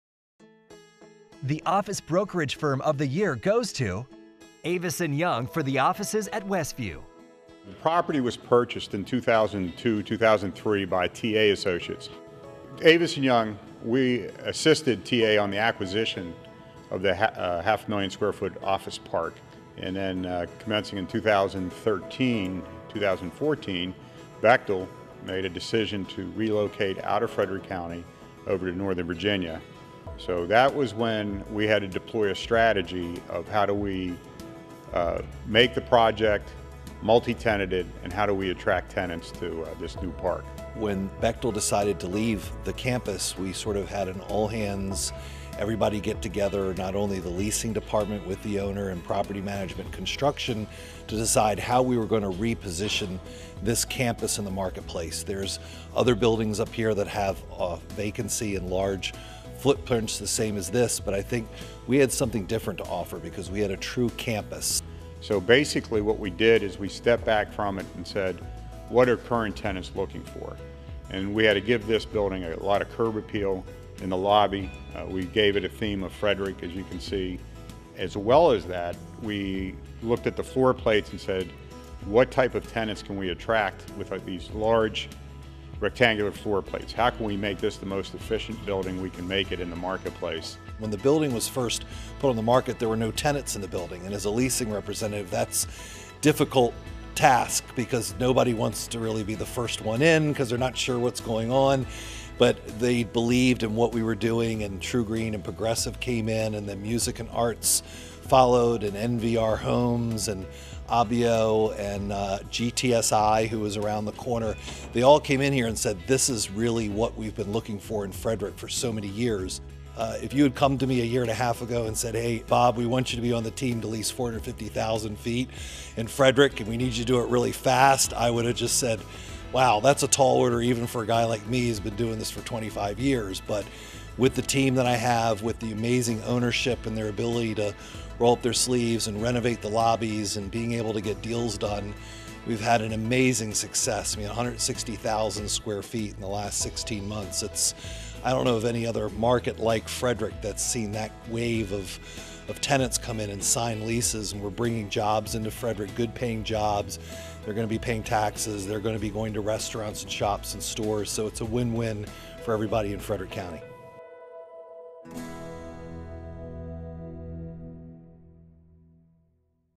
2017 FRED Awards Office Brokerage Firm of the Year: Avison Young
The Office Brokerage Firm of the Year came down to one firm for their vision to transform what was once a large corporate campus into a home for several tenants. As the demand for larger offices shrinks and technology allows us to need less space, this firm responded to office trends in commercial real estate with a product that is attractive and modern.